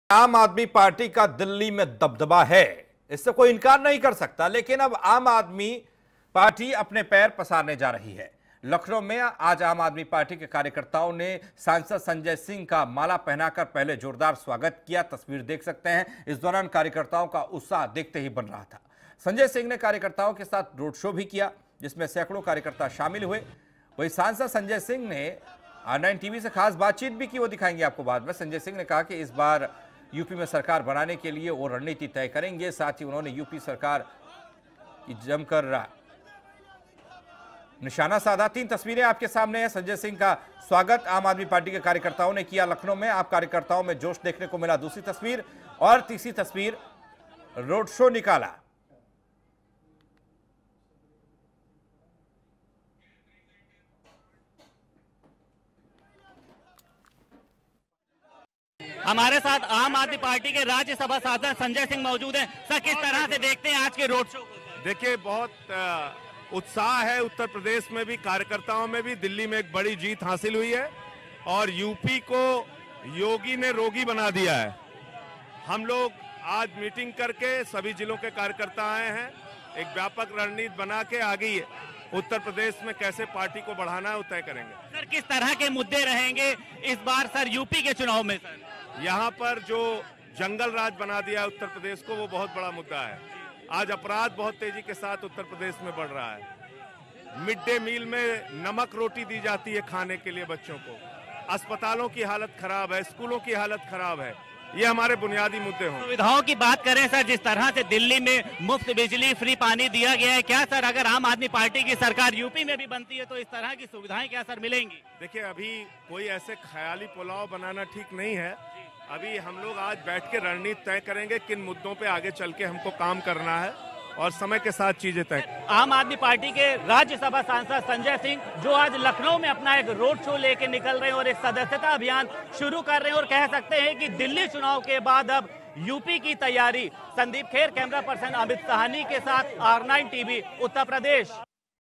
लखनऊ: आप की नज़र अब यूपी पर, सांसद ने निकाला विजय जुलूस
संजय सिंह ने कार्यकर्ताओं के साथ रोड शो भी किया जिसमें सैकड़ों कार्यकर्ता शामिल हुए.संजय सिंह ने कहा कि इस बार यूपी में सरकार बनाने के लिए वो रणनीति तय करेंगे साथ ही उन्होंने यूपी सरकार भी जमकर निशाना साधा है.